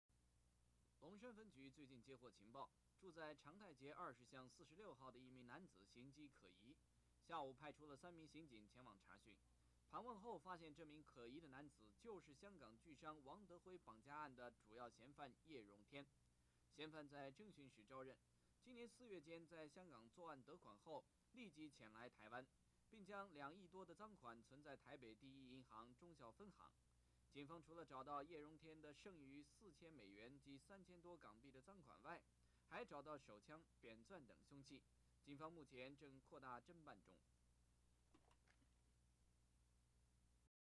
【歷史上的今天】1983.09.24_香港巨商王德輝綁架案嫌犯在台落網
龍山分局最近接獲情報，住在長泰街20巷46號的一名男子形跡可疑，下午派出了三名刑警前往查詢，盤問後發現這名可疑的男子就是香港富商王德輝綁架案的主要嫌犯葉榮添。